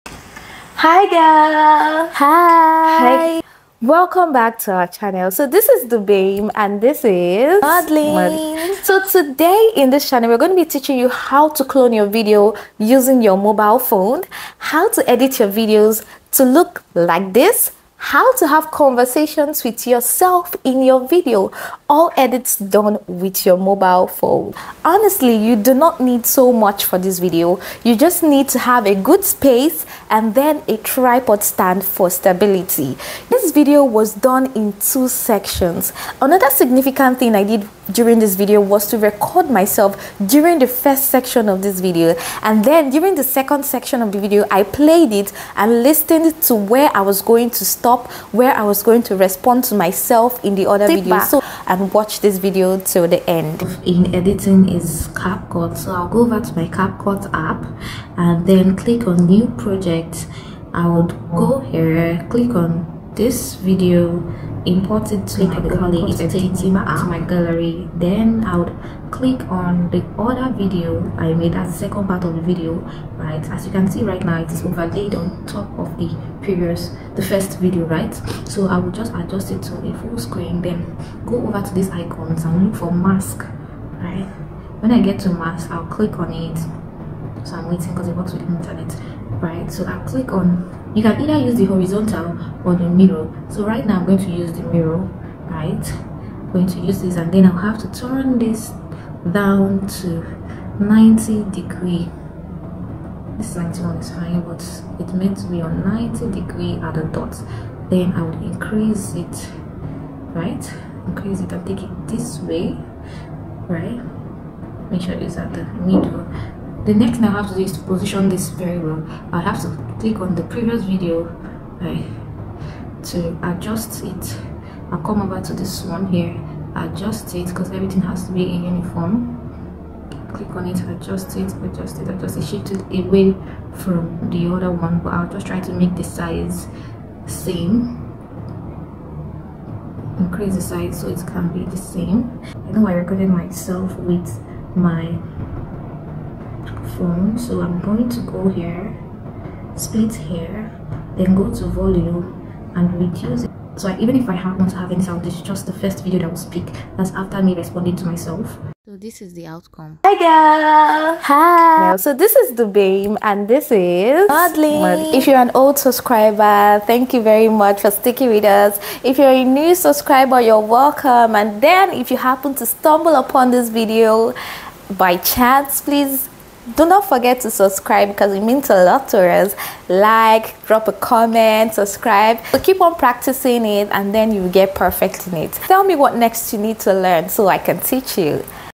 HOW TO FILM AND EDIT A CONVERSATION WITH YOURSELF USING YOUR SMARTPHONE| video editing
Happy to be fully back on YouTube.

In this tutorial I will be teaching you how to clone yourself in a video and have conversations with yourself on your mobile phone using capcut app.

This is a detailed tutorial on how to givenyour audience a good show.

IF THIS VIDEO  WAS HELPFUL  PLEASE LET ME KNOW DOEN IN THE COMMENT SECTION

 LINK TO DOWNLOAD CAPCUT APP